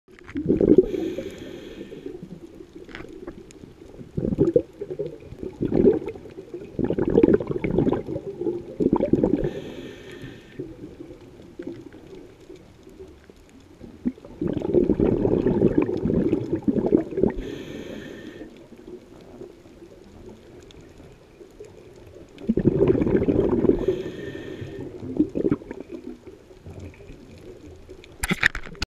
Go to School_Wreck of Cali
Lionfish culling tournament dive for CULL12 but we paused to watch this school of fish.